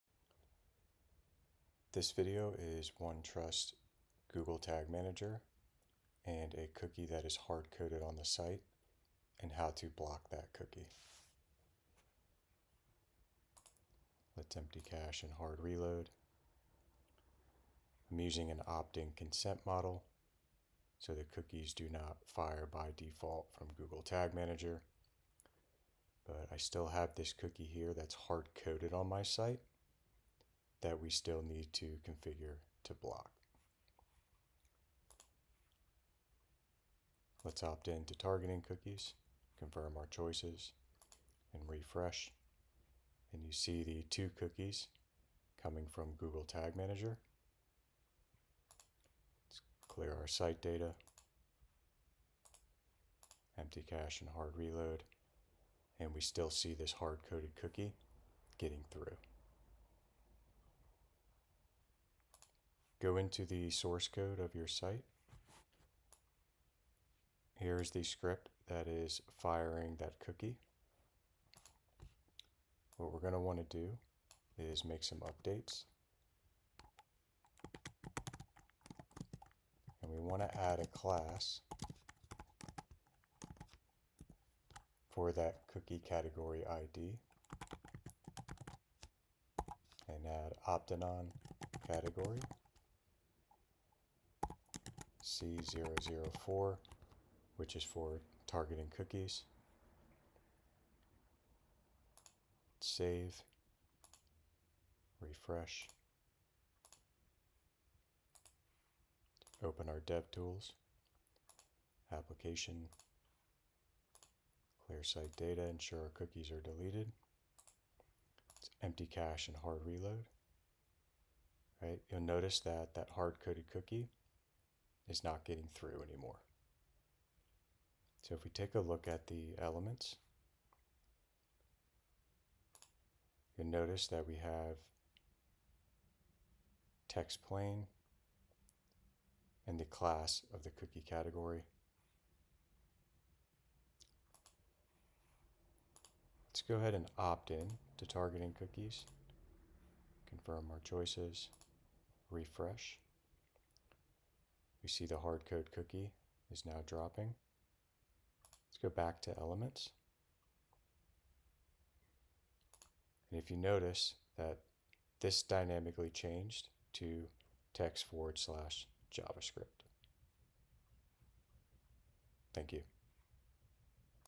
OneTrust Google Tag Manager and Blocking Hard Coded Cookie
Video describing OneTrust, Google Tag Manager, and how to also block a cookie that is hard coded on the website.